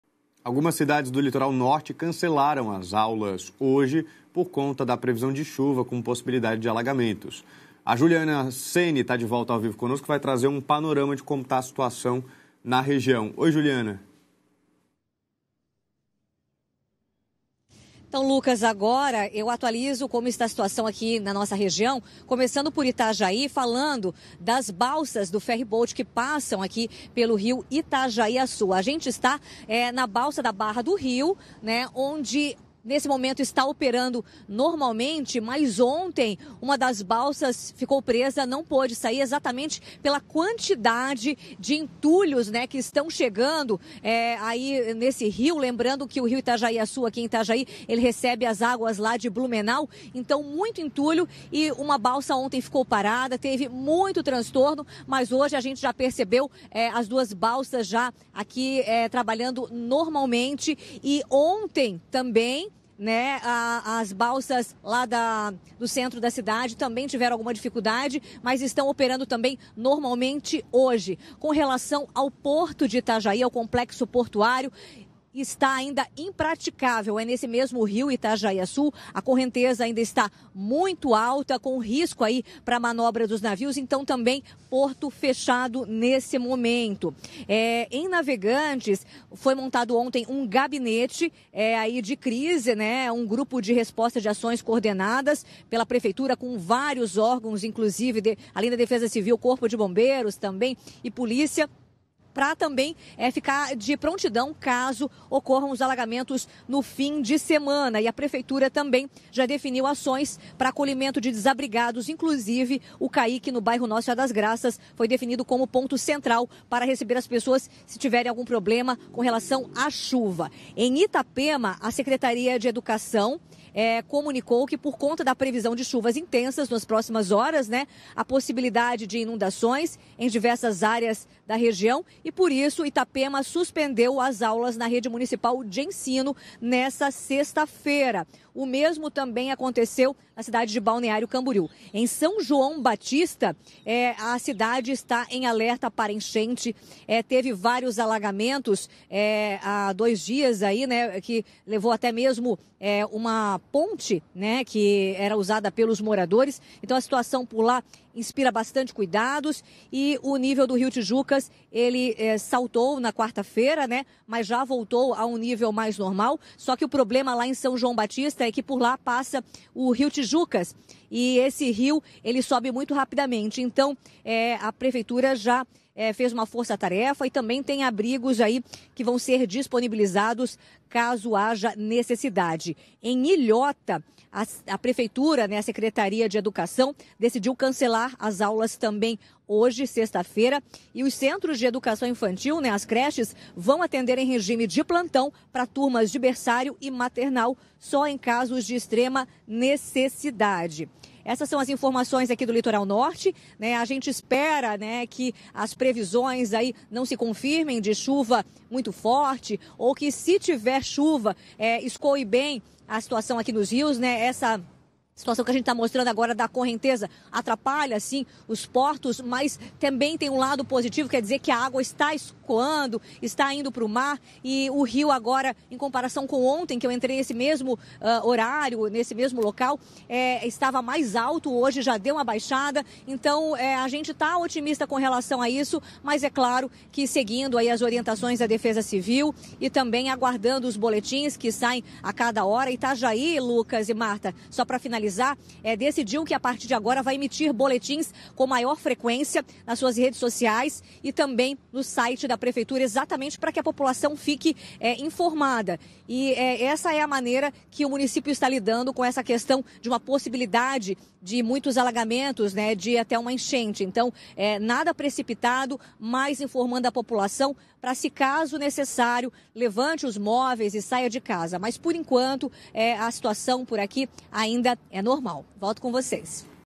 Chuva intensa: cidades do Litoral Norte cancelam aulas nesta sexta-feira
A decisão prevê garantir a segurança da comunidade escolar da cidade diante do atual cenário de chuvas na região. Confira.